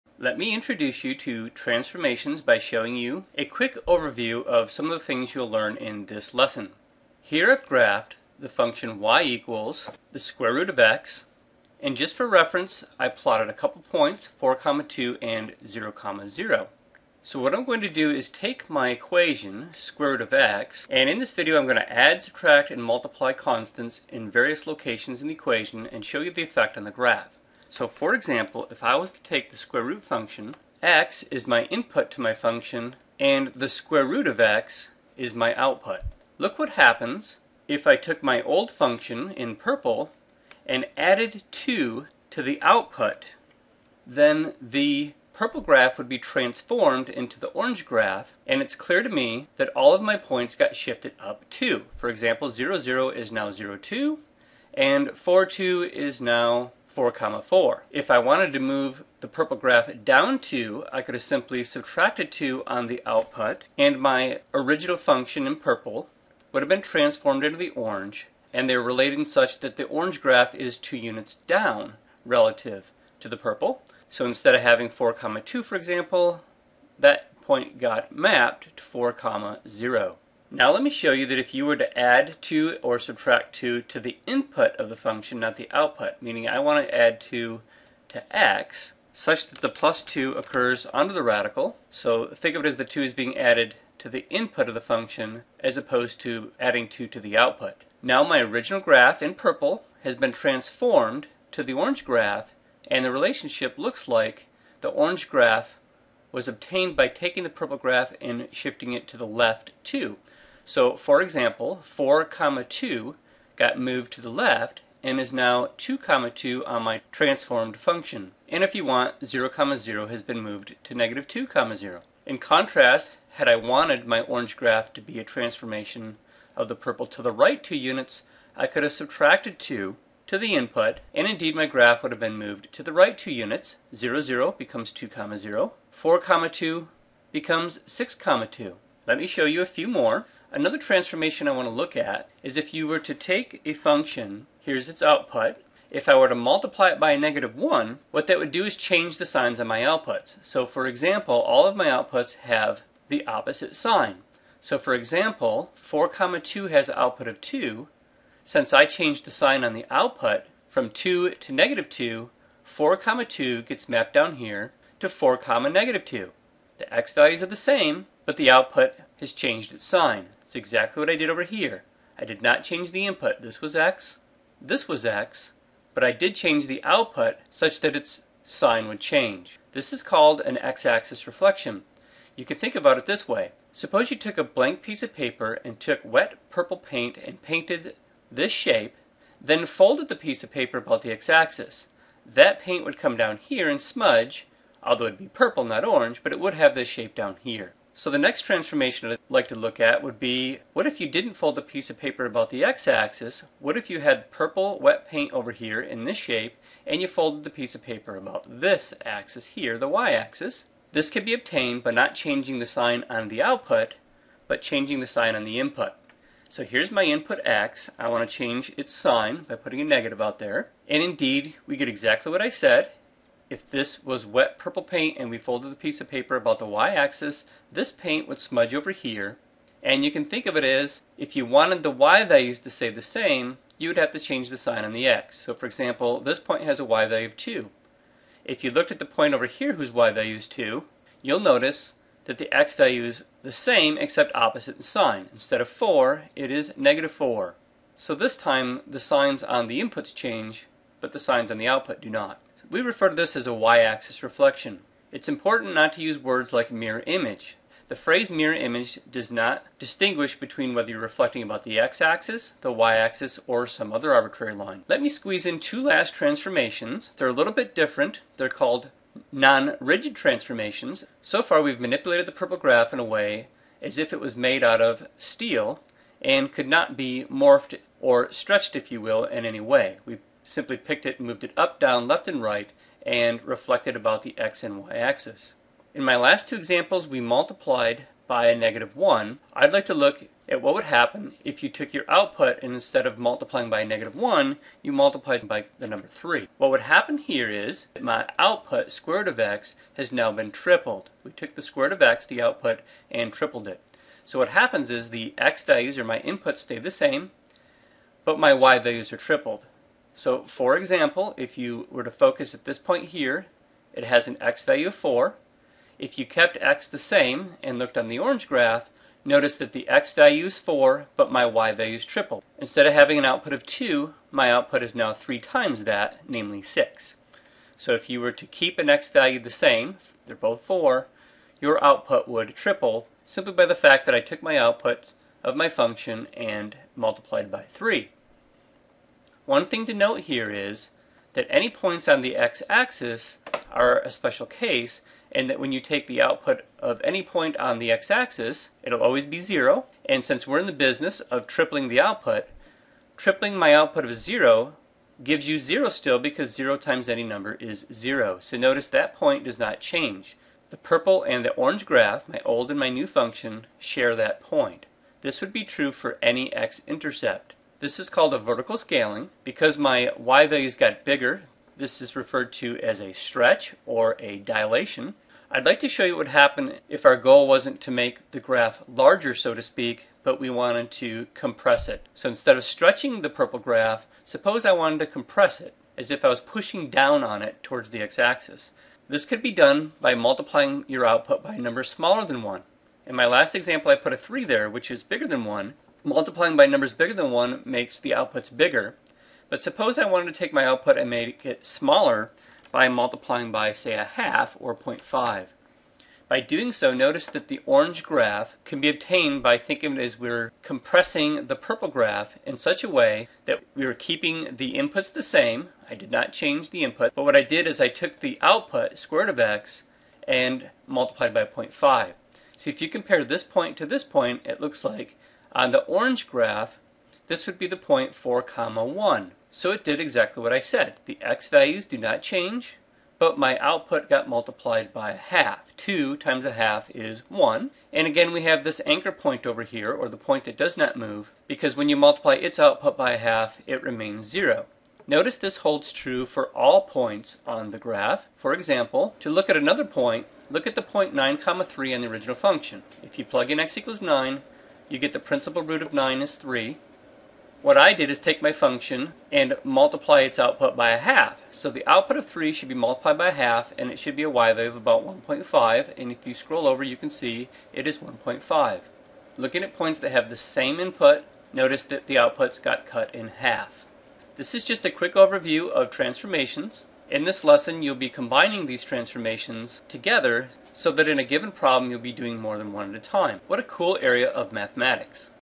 An Overview of Trasformations
This video shows 8 basic transformations on functions.